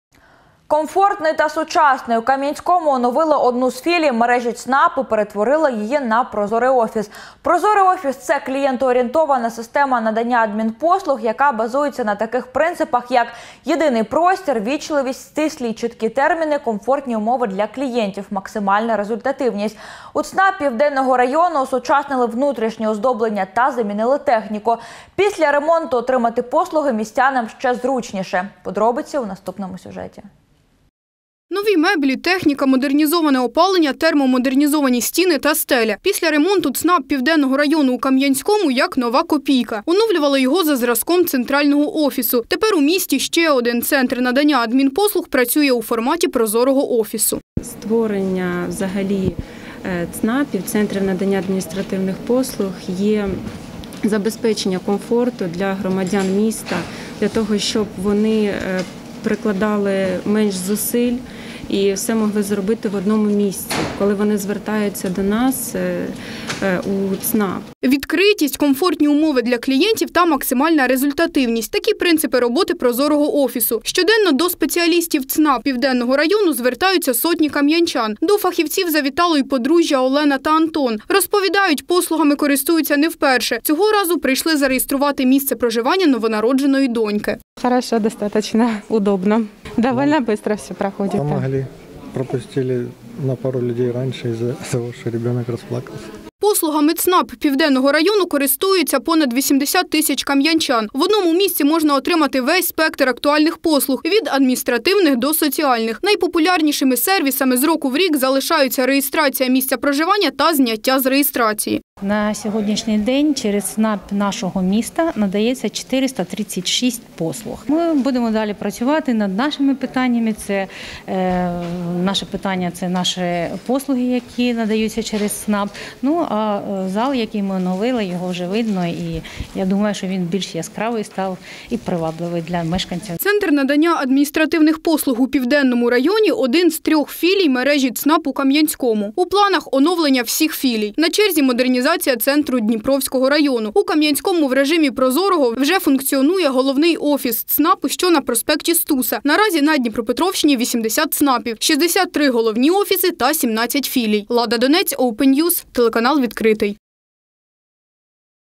“Прозорий офіс”: у Кам'янському оновили ЦНАП Південного району
Комфортний та сучасний. У Кам’янському оновили одну з філій мережі ЦНАП і перетворили її на “прозорий офіс”. Прозорий офіс – це клієнтоорієнтована система надання адмінпослуг, яка базується на таких принципах, як єдиний простір, ввічливість, стислі й чіткі терміни, комфортні умови для клієнтів, максимальна результативність. У Центрі надання адмінпослуг Південного району замінили внутрішнє оздоблення та осучаснили техніку. Після ремонту отримати послуги містянам ще зручніше _______________________________________________